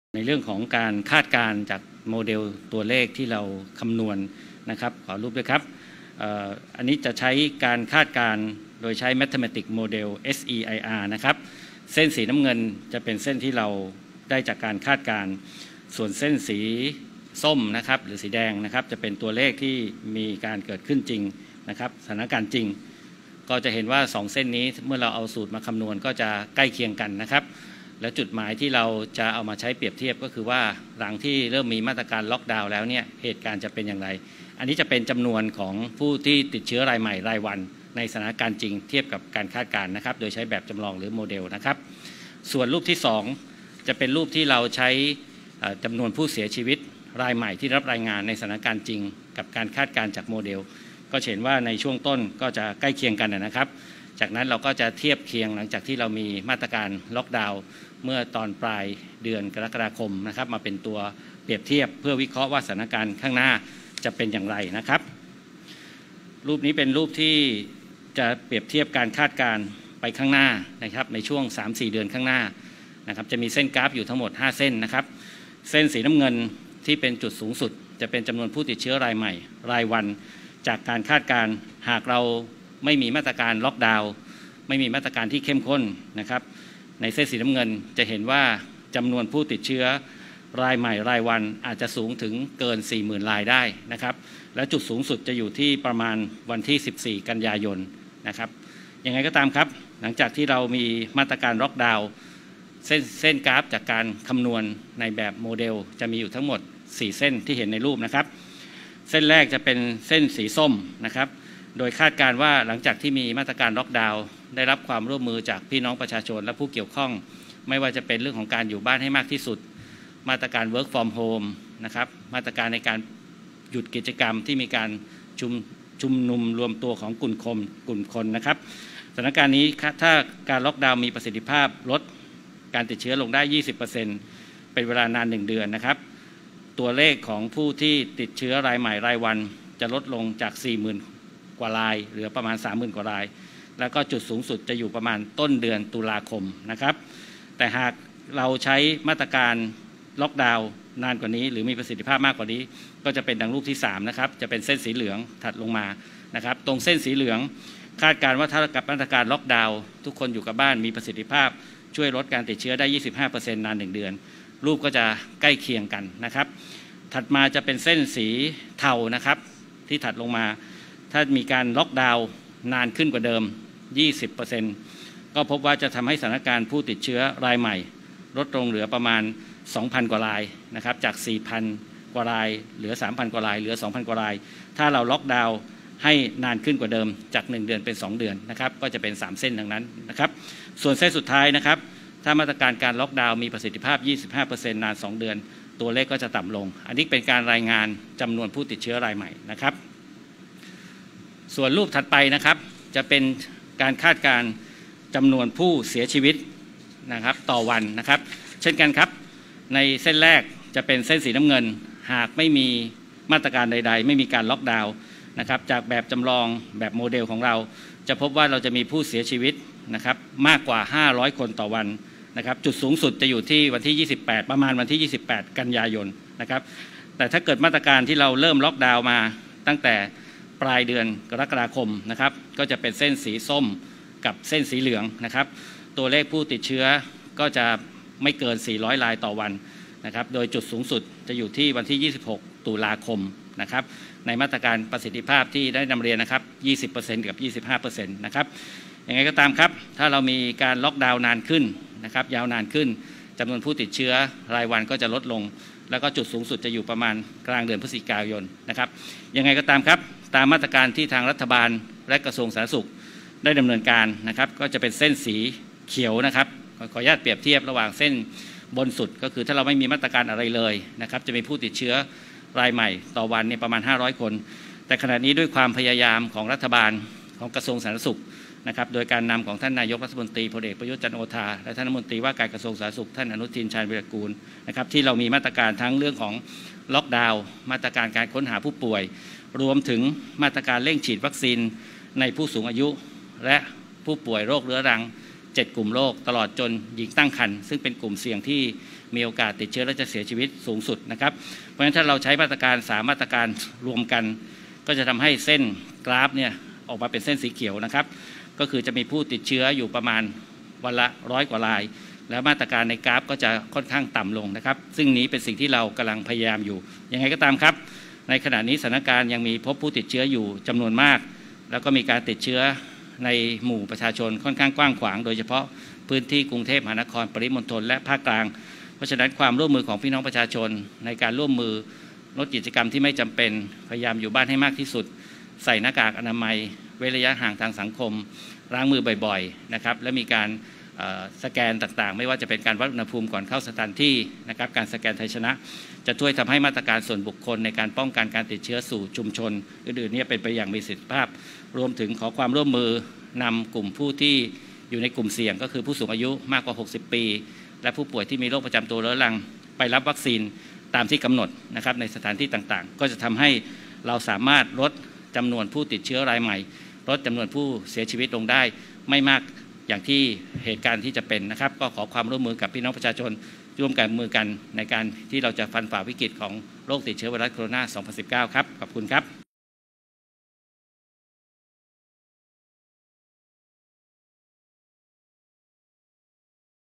สธ. คาด เดือนตุลาคม ผู้ติดเชื้อจะพุ่งสูงสุด ล็อกดาวน์ ต่อ 2 เดือน ลดตาย-ติดเชื้อ | workpointTODAY
วันที่ 30 ก.ค. 2564 นพ.โอภาส การย์กวินพงศ์ อธิบดีกรมควบคุมโรค กล่าวถึงการคาดการณ์ตัวเลขผู้ติดเชื้อและผู้เสียชีวิตโดยใช้แบบจำลองทางคณิตศาสตร์ SCIR ว่า ในสถานการณ์ปัจจุบัน การล็อกดาวน์มีประสิทธิภาพช่วยลดจำนวนผู้ติดเชื้อได้ร้อยละ 20 โดยในระยะเวลาการล็อกดาวน์ 1 เดือน

คาดจำนวนผู้ติดเชื้อจำนวนสูงสุดจะอยู่ในช่วงต้นเดือน ต.ค. ที่ 35,000 รายต่อวัน แต่ถ้าขยายเวลาล็อกดาวน์เป็น 2 เดือน จำนวนผู้ติดเชื้อคาดว่าจะมากที่สุดในต้นเดือน พ.ย. ที่ 25,000 รายต่อวัน

ทางด้านของจำนวนผู้เสียชีวิต ในสถานการณ์ปัจจุบัน ด้วยการล็อกดาวน์ระยะเวลา 1 เดือน คาดมีผู้เสียชีวิตสูงสุดวันที่ 26 ต.ค. เกือบ 500 รายต่อวัน และหากขยายเวลาล็อกดาวน์เป็น 2 เดือน ผู้เสียชีวิตสูงสุดจะอยู่ในช่วงสิ้นเดือน พ.ย. ที่ 350 รายต่อวัน
ทั้งนี้ คาดว่า ด้วยมาตรการของกระทรวงสาธารณสุขในการล็อกดาวน์ระยะเวลา 2 เดือน รวมกับการค้นหาผู้ป่วย และเร่งฉีดวัคซีนในกลุ่มเสี่ยง จำนวนผู้เสียชีวิตสูงสุดจะไม่เกิน 200 รายต่อวัน

พร้อมขอความร่วมมือประชาชนลดกิจกรรมที่ไม่จำเป็น ลดการออกจากบ้าน ใส่หน้ากากอนามัย ลดระยะห่างทางสังคม และพากลุ่มเสี่ยงเข้ารับวัคซีนเพื่อลดจำนวนผู้ติดเชื้อรายใหม่ให้จำนวนน้อยกว่าการคาดการณ์ดังกล่าว